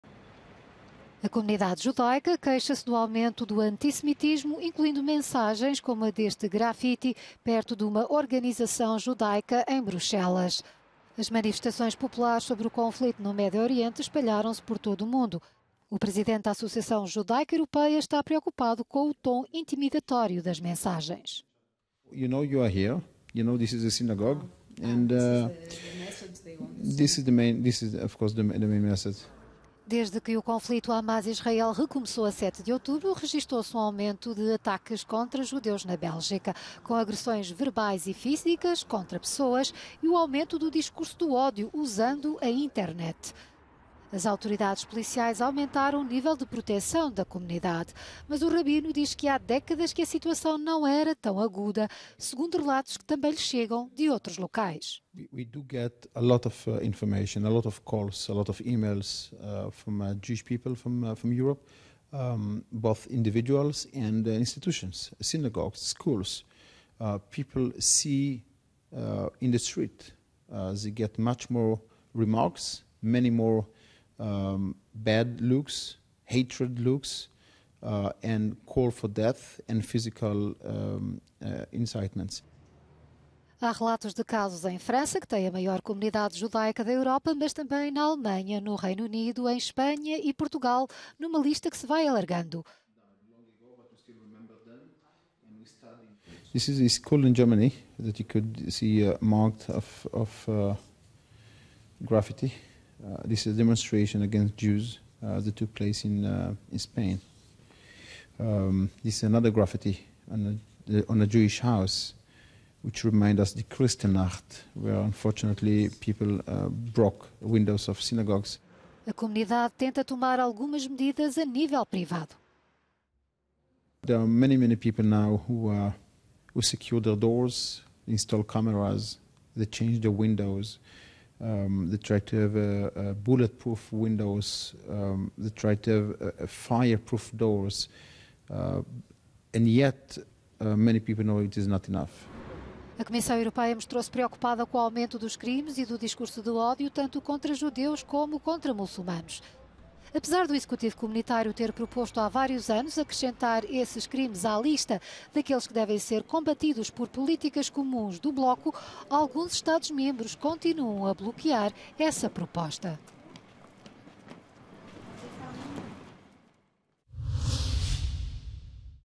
Associação Judaica Europeia alerta para antissemitismo
As manifestações populares sobre o conflito no Médio Oriente espalharam-se por todo o mundo e o presidente da Associação Judaica Europeia, Menachem Margolin, está preocupado com as mensagens intimidatórias.